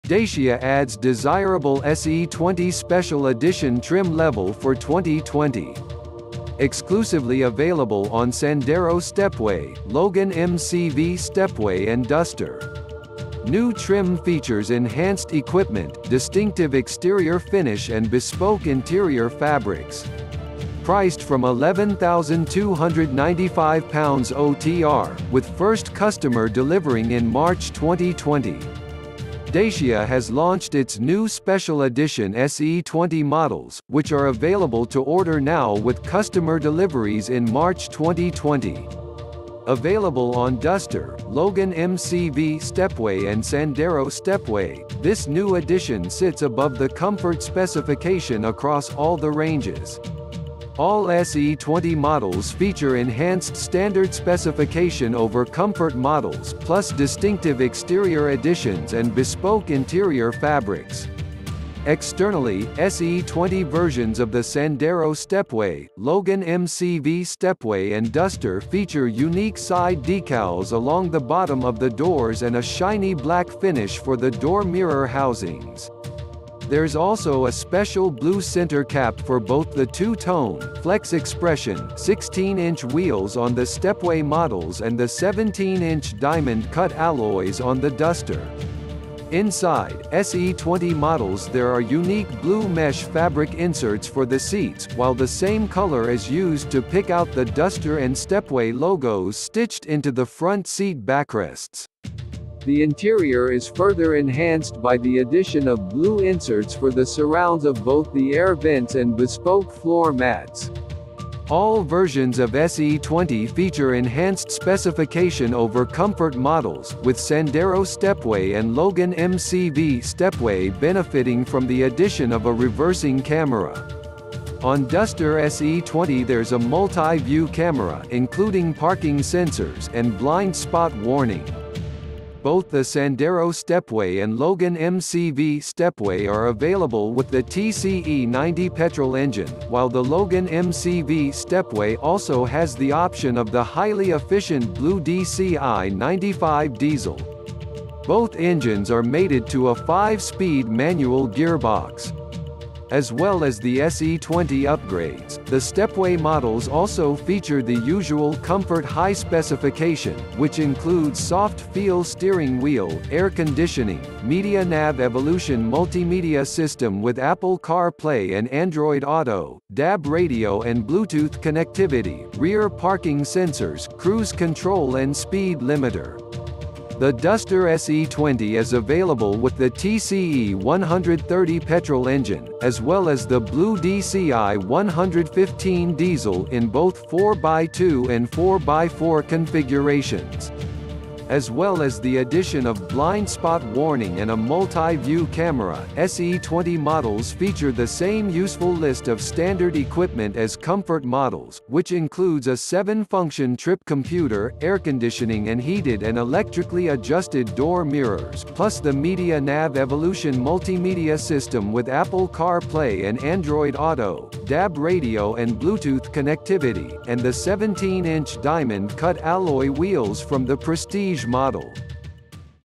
New Decade, New Dacia. Explore the 2020 Special Editions.
Dacia adds desirable S.E Twenty special edition trim level for 2020.
Exclusively available on Sandero Stepway, Logan MCV Stepway and Duster.
New trim features enhanced equipment, distinctive exterior finish and bespoke interior fabrics.
Priced from £11,295 OTR, with first customer delivering in March 2020.
Available on Duster, Logan MCV Stepway and Sandero Stepway, this new addition sits above the Comfort specification across all the ranges. All SE Twenty models feature enhanced standard specification over Comfort models, plus distinctive exterior additions and bespoke interior fabrics.
Externally, S.E Twenty versions of the Sandero Stepway, Logan MCV Stepway and Duster feature unique side decals along the bottom of the doors and a shiny black finish for the door mirror housings. There’s also a special blue centre cap for both the two-tone ‘Flex Expression’ 16-inch wheels on the Stepway models and the 17-inch diamond cut alloys on the Duster.
Inside, S.E Twenty models there are unique blue mesh fabric inserts for the seats, while the same colour is used to pick out the Duster and Stepway logos stitched into the front seat backrests. The interior is further enhanced by the addition of blue inserts for the surrounds of both the air vents and bespoke floor mats.
All versions of S.E Twenty feature enhanced specification over Comfort models, with Sandero Stepway and Logan MCV Stepway benefitting from the addition of a reversing camera. On Duster SE Twenty there’s a multi-view camera (including parking sensors) and Blind Spot Warning.
Both the Sandero Stepway and Logan MCV Stepway are available with the TCe 90 petrol engine, while the Logan MCV Stepway also has the option of the highly efficient Blue dCi 95 diesel. Both engines are mated to a five-speed manual gearbox.
As well as the SE Twenty upgrades, the Stepway models also feature the usual Comfort high specification, which includes soft-feel steering wheel, air conditioning, Media Nav Evolution multimedia system with Apple CarPlay® and Android Auto®, DAB Radio and Bluetooth connectivity, rear parking sensors, cruise control and speed limiter.
The Duster S.E Twenty is available with the TCe 130 petrol engine, as well as the Blue dCi 115 diesel in both 4x2 and 4x4 configurations.
As well as the addition of Blind Spot Warning and a multi-view camera, SE Twenty models feature the same useful list of standard equipment as Comfort models, which includes a 7-function trip computer, air-conditioning and heated and electrically adjusted door mirrors, plus the Media Nav Evolution multimedia system with Apple CarPlay® and Android Auto®, DAB Radio and Bluetooth connectivity, and the 17-inch Diamond cut alloy-wheels from the Prestige model. Do you drive a Dacia? Are they worth every penny or do you get what you pay for?

MotoHQ, The home for Petrolheads